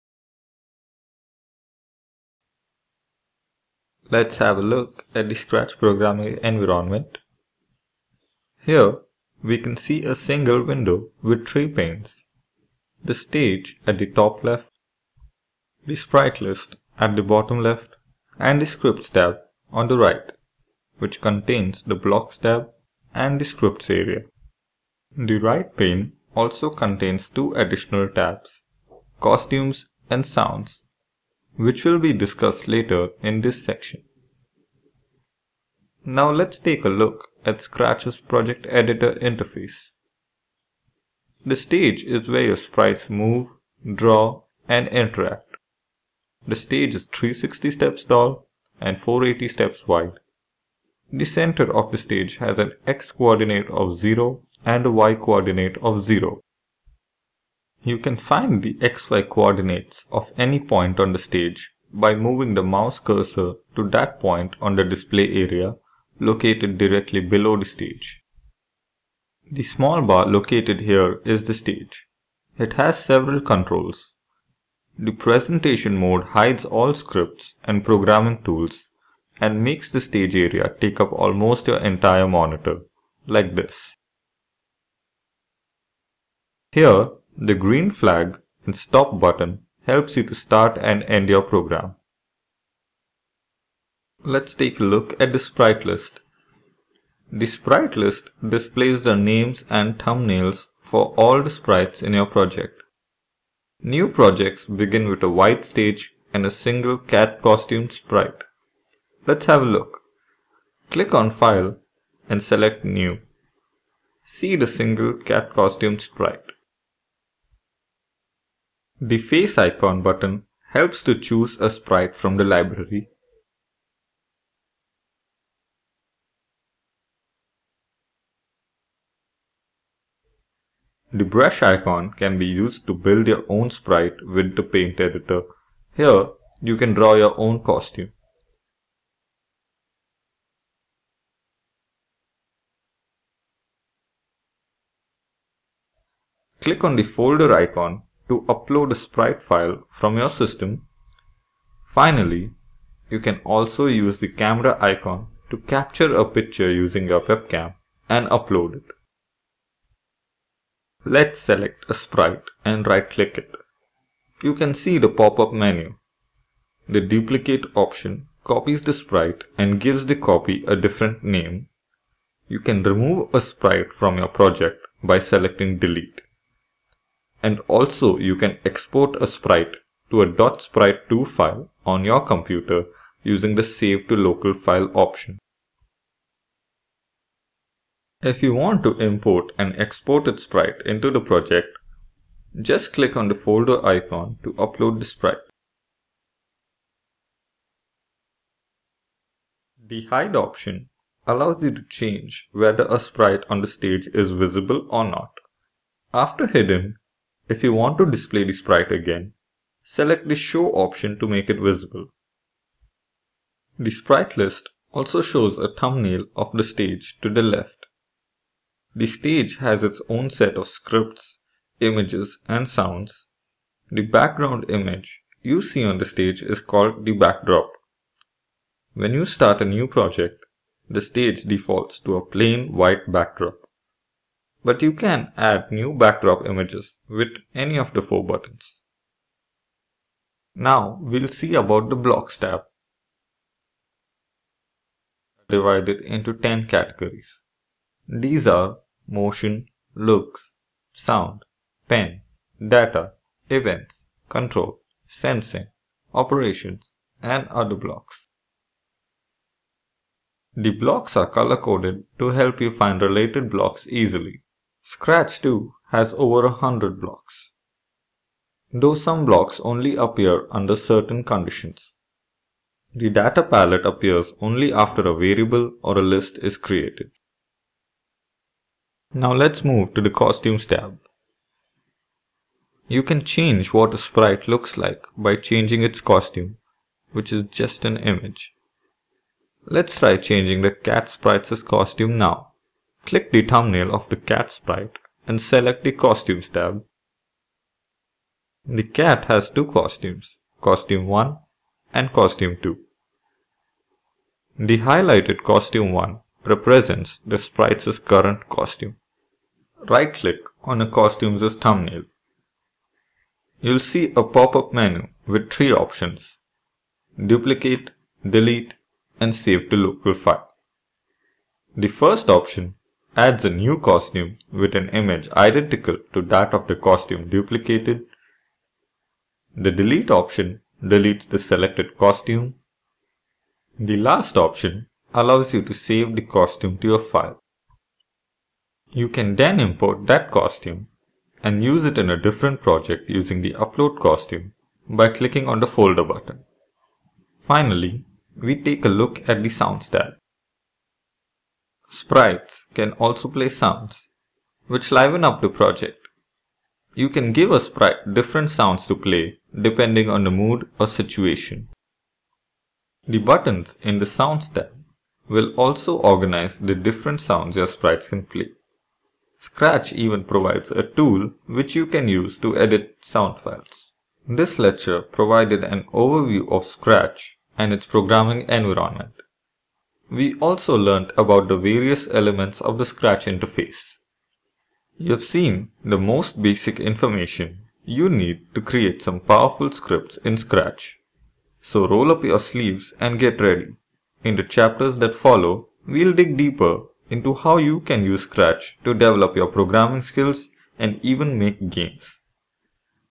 Scratch Programming Environment
In this video, you will get to know the basics of the Scratch editor interface. You will learn about the Stage, Sprite, Sprite List, Scripts Tab, Costumes and the Sounds Tab. Specifically, you will learn about the Stage, where you work on your project. You will also learn about Sprites and the Sprites List. Next, you will learn about the Scripts Tab, Costumes and the Sounds Tab.